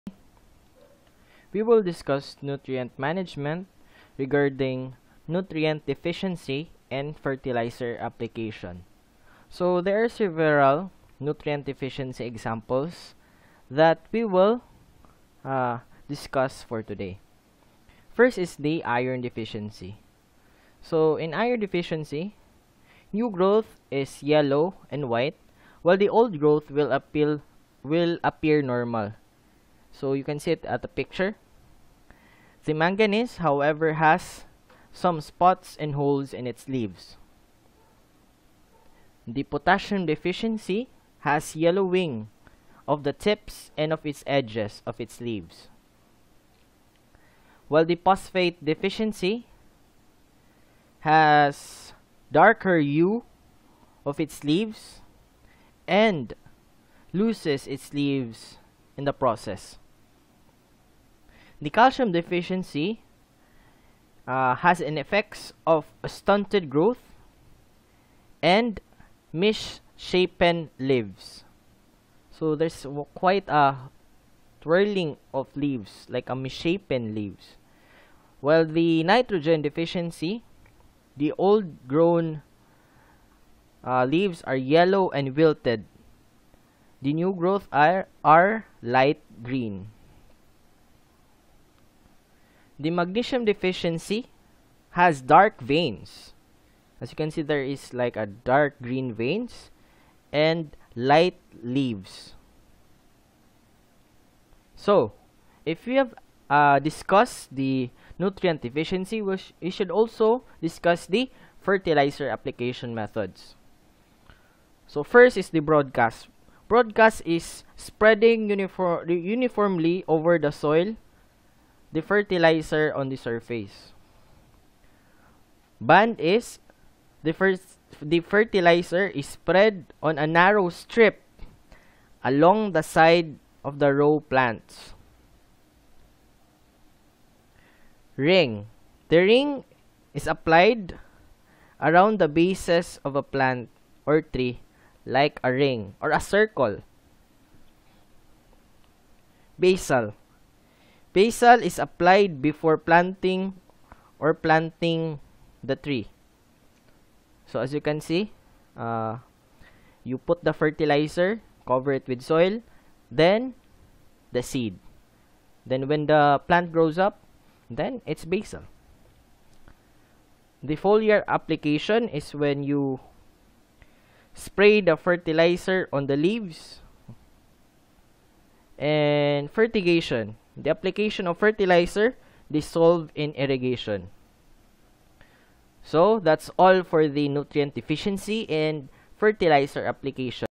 How to Identify Nutrient Deficiency and Fertilizer Application
Farmers must know what are the signs that their crops are nutrient deficient. It is also important to know what are available ways of applying fertilizer to have better yields.